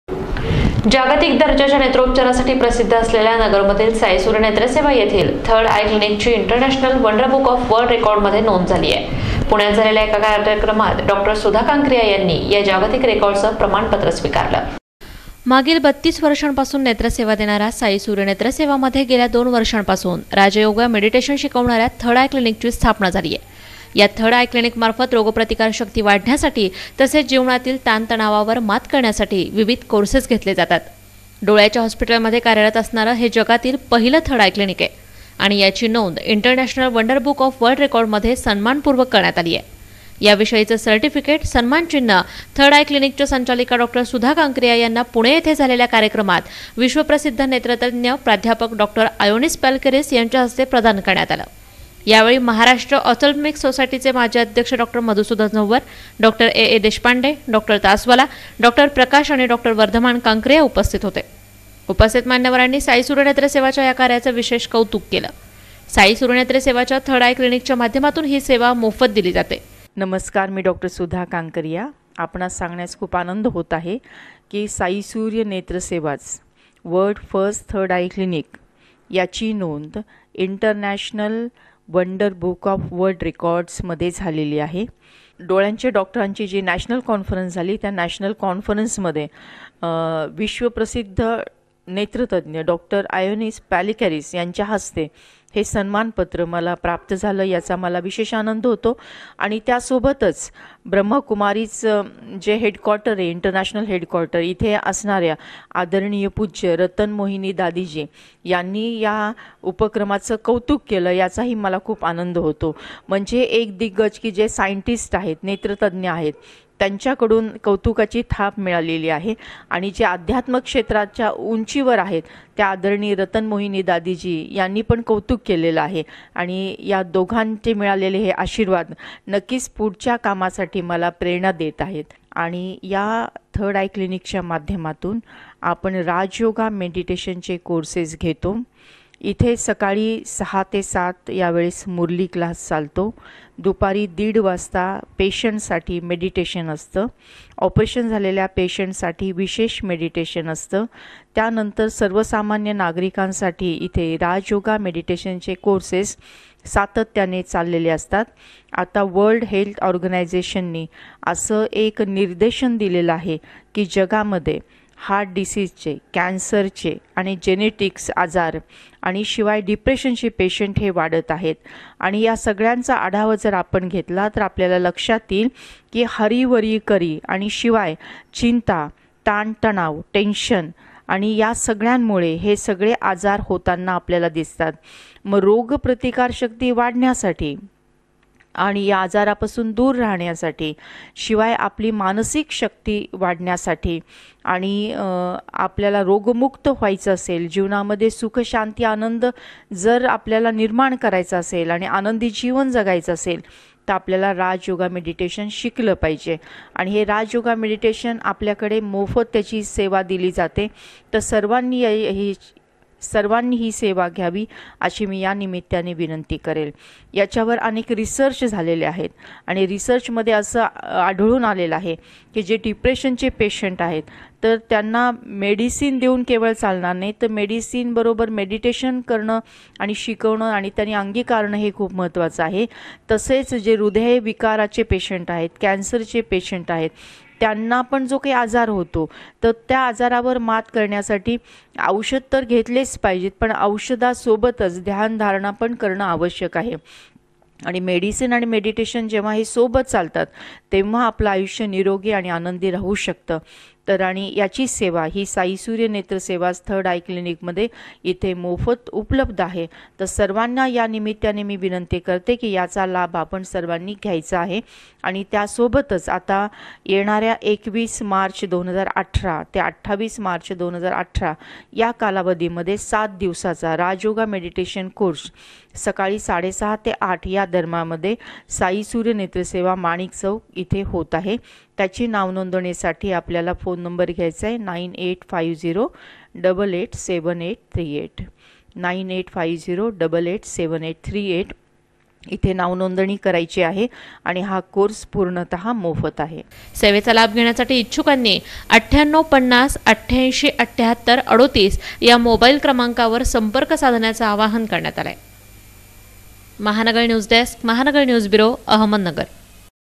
महानगर न्यूज - साई सूर्य नेत्रसेवातील थर्ड आय क्लिनिकची वर्ल्ड रेकॉर्डमध्ये नोंद
मेडिटेशनच्या माध्यमातून चालवलेल्या कार्याचा जागतिक गौरव